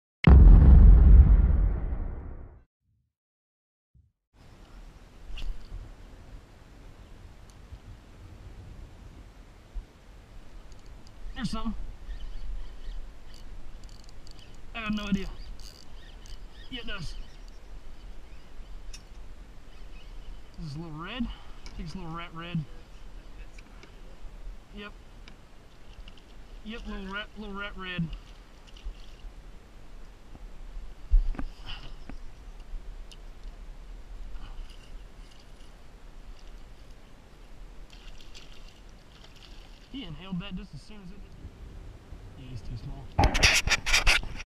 Small redfish
Caught a small red "rat red" in the flats of Boca Grande Fl.